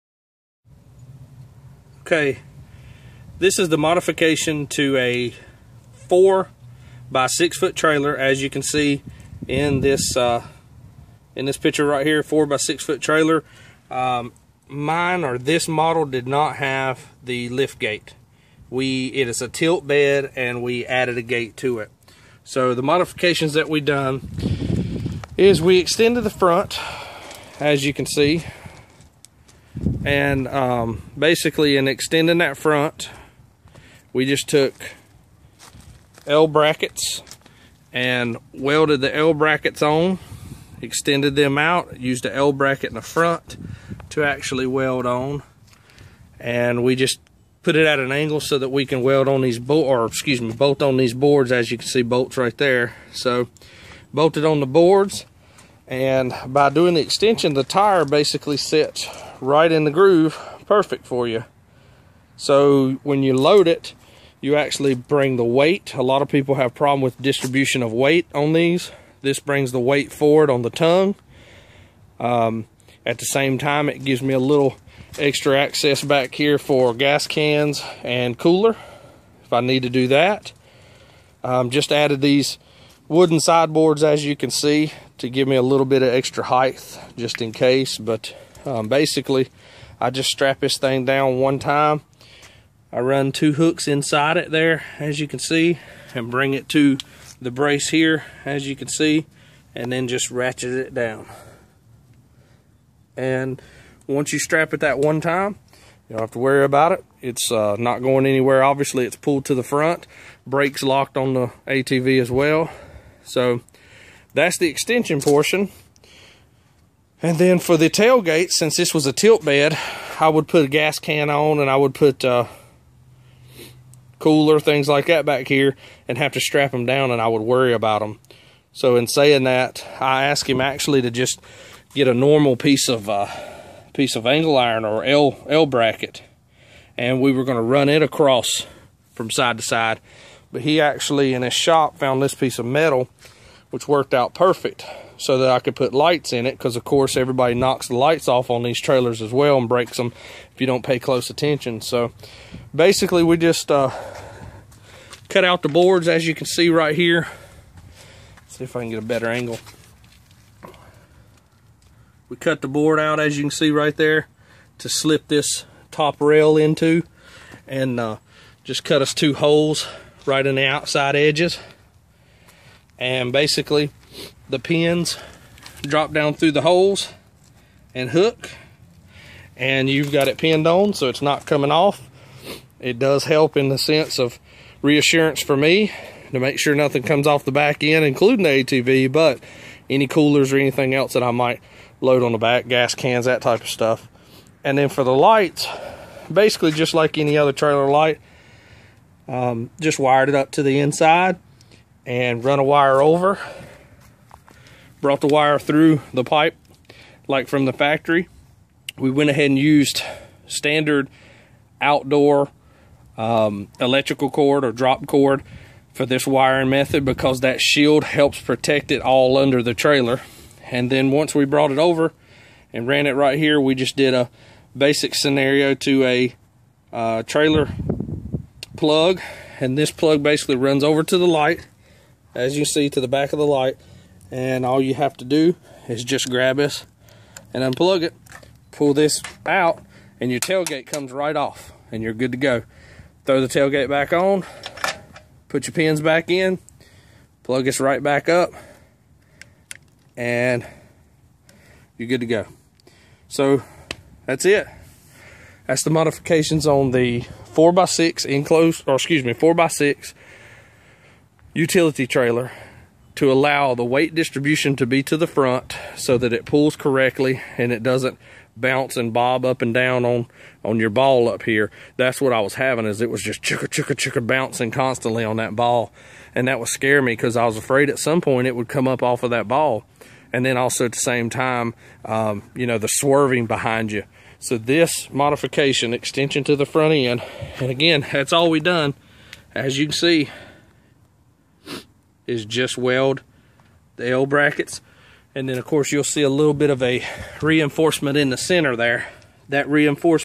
Custom 4x6 utility atv trailer
This is a custom modified 4x6 tilt bed utility trailer. It is custom made for an ATV. This is a great way to customize your trailer for a TV. Theres also a great tailgate invention!